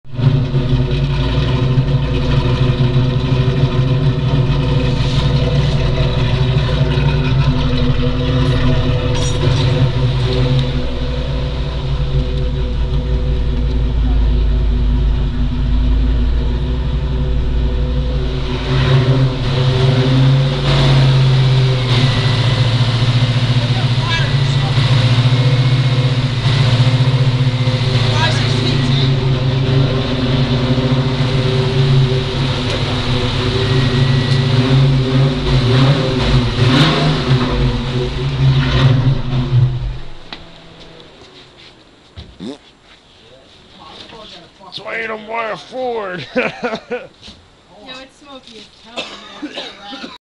wallys car first start up take 3
wallys 302 stroker mustang engine first start up